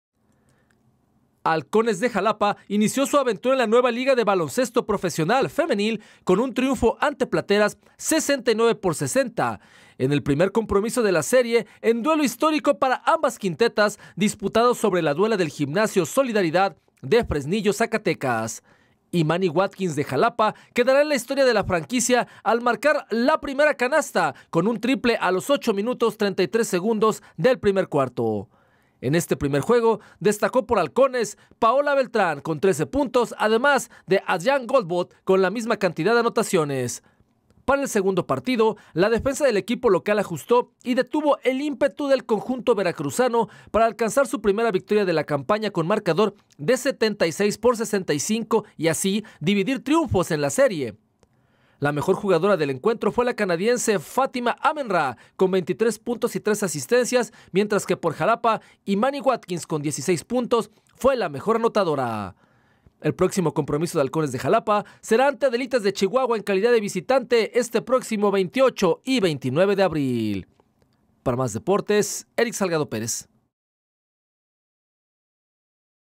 HALCONES DE XALAPA FEM VS PLATERAS DE FRESNILLO
El equipo de Básquetbol Halcones de Xalapa Femenil y Plateras de Fresnillo competirán en la temporada de la Liga Nacional de Baloncesto Profesional Femenil.